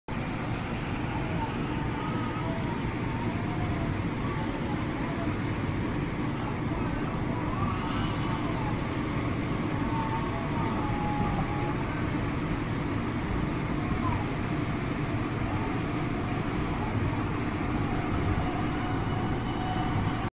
Fresh from the manufacturers
Two  brand new Kinki Sharyos for the Metro Gold Line at the DIV 34 yard in Monrovia!